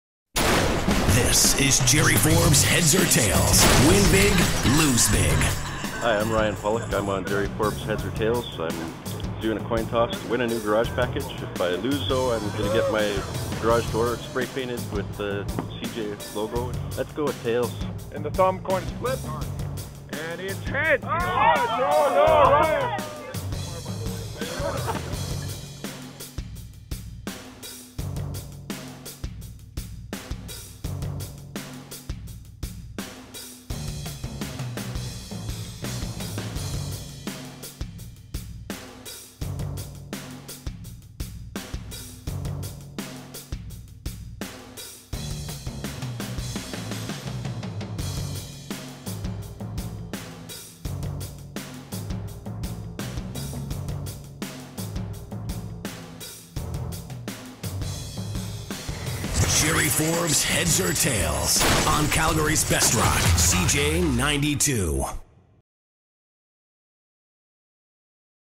Gerry Forbes Heads or Tails Toss #2: Garage Reno
Ryan LOST Heads or Tails Coin Toss #2 so we graffiti'd his garage door with the CJAY 92 logo!